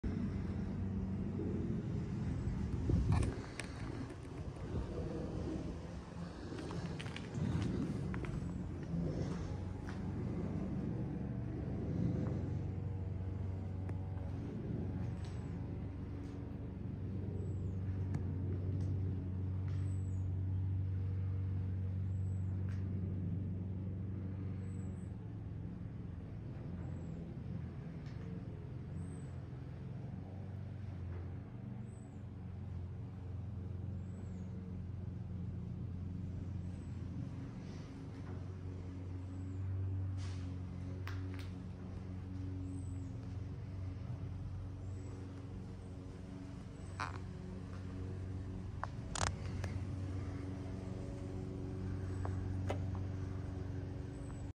22 August 2024
Fire extinguishers for the Twinwoods Festival in the UK which will help keep everyone safe 🧯👑🔥